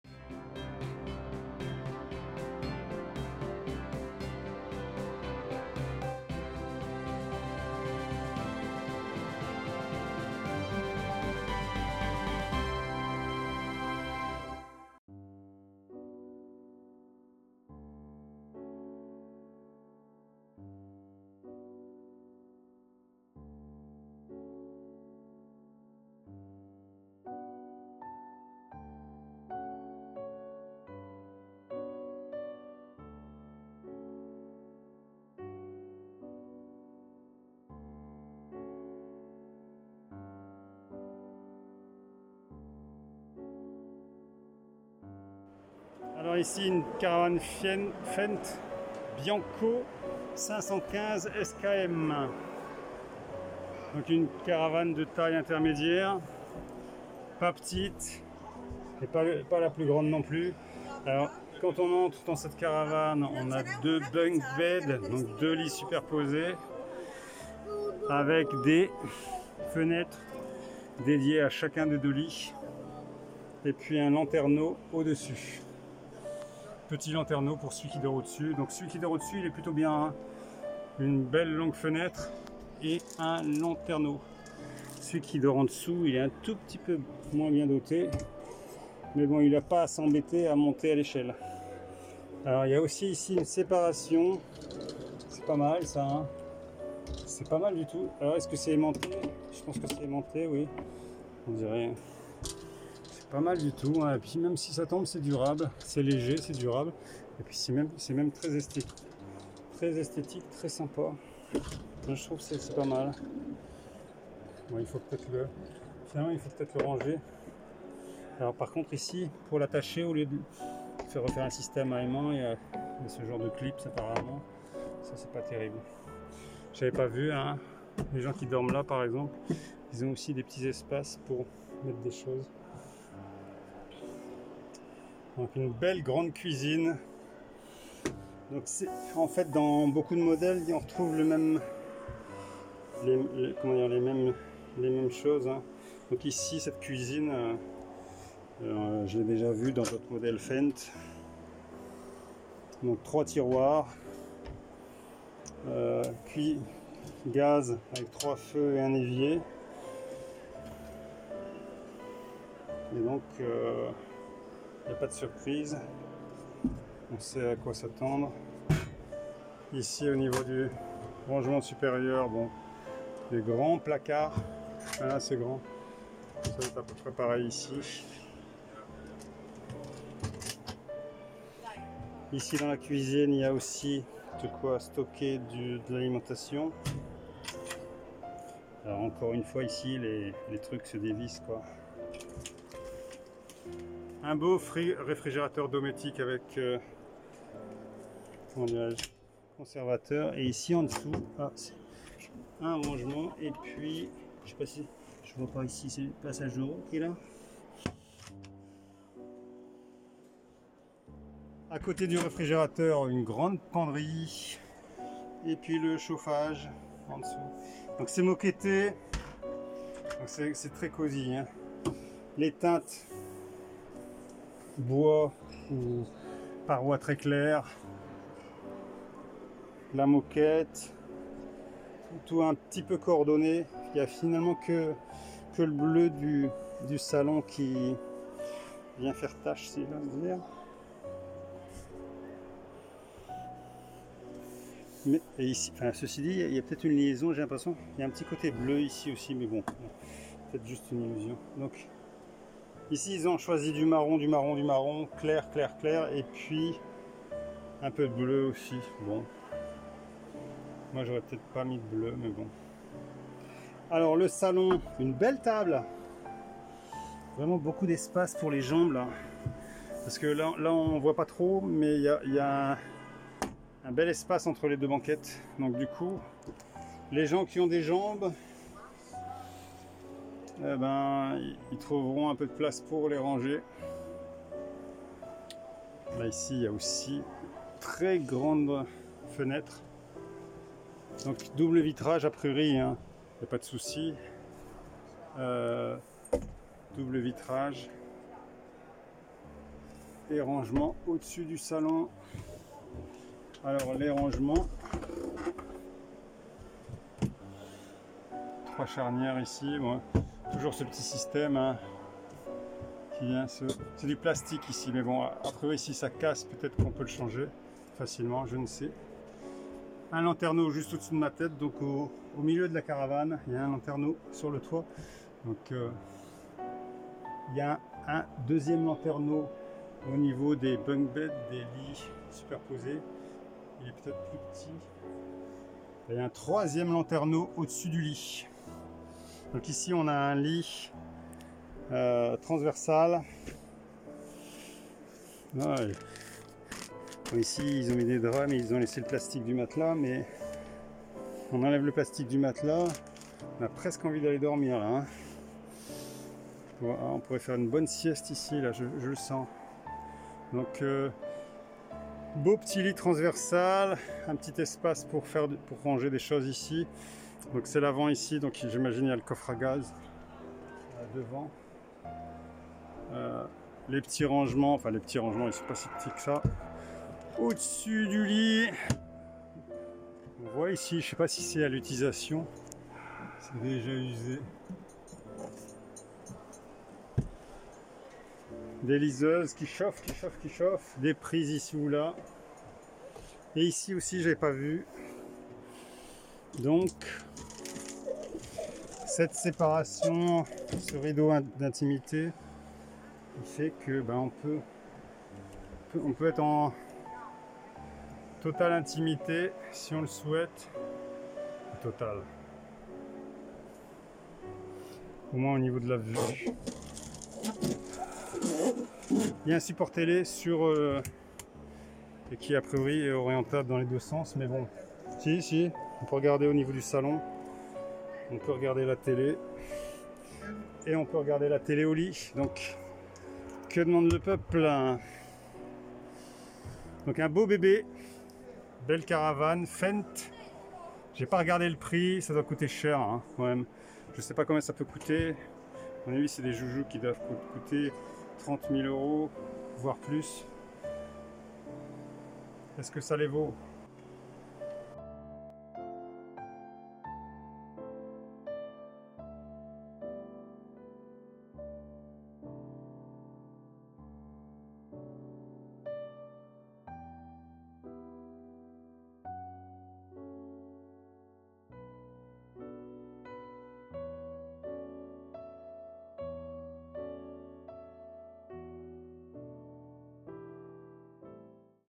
FENDT Bianco Selection 515 SKM - Parma 2021 - Salone del Camper (45)
A useful 6.42 meter caravan, slightly larger than the previous one.
But this one for 5 people! In other words, almost everything can be transformed into a bed! Two bunk beds, a two person front bed and a single dinette bed! For the rest, there is no shortage of space to take the meal or store the luggage of this large assembly. Everything is well designed.
Except for the bathroom, identical to that of the previous Saphir model!
There, it's really for amateurs... You be the judge!

Ein nützlicher 6,42-Meter-Wohnwagen, etwas größer als der vorherige.
Aber dieses für 5 Personen! Mit anderen Worten, fast alles kann in ein Bett verwandelt werden! Zwei Etagenbetten, ein Zwei-Personen-Frontbett und ein Ess-Einzelbett! Im Übrigen fehlt es nicht an Platz, um das Essen einzunehmen oder das Gepäck dieser großen Versammlung aufzubewahren. Alles ist gut gestaltet.
Bis auf das Bad identisch mit dem des Vorgängermodells Saphir!
Dort ist es wirklich für Amateure ... Sie sind der Richter!

Un utile caravan di 6,42 metri, leggermente più grande del precedente.
Ma questo per 5 persone! In altre parole, quasi tutto può essere trasformato in un letto! Due letti a castello, un letto anteriore per due persone e un letto singolo in dinette! Per il resto, lo spazio non manca per consumare il pasto o riporre i bagagli di questo grande raduno. Tutto è ben progettato.
Fatta eccezione per il bagno, identico a quello del precedente modello Saphir!
Ecco, è davvero per dilettanti... Sii tu il giudice!

Una caravana útil de 6,42 metros, algo más grande que la anterior.
Pero este para 5 personas! En otras palabras, ¡casi todo se puede transformar en una cama! ¡Dos literas, una cama frontal para dos personas y una cama de comedor individual! Por lo demás, no falta espacio para tomar la comida o guardar el equipaje de este gran conjunto. Todo está bien diseñado.
Excepto el baño, ¡idéntico al del modelo Saphir anterior!
Ahí, es realmente para aficionados... ¡Sea usted el juez!

Une caravane de 6,42 mètres utiles, un peu plus grande que la précédente.
Mais celle -ci pour 5 personnes ! Autant dire que tout ou presque, peut se transformer en lit ! Deux lits superposés, un lit avant de deux personnes et un lit dinette simple ! Pour le reste, il ne manque pas de place pour prendre le repas ou ranger les bagages de cette nombreuse assemblée. Tout est bien conçu.
Sauf la salle de bain, identique à cette du modèle Saphir précédent !
Là, c'est vraiment pour amateurs ... A vous de juger !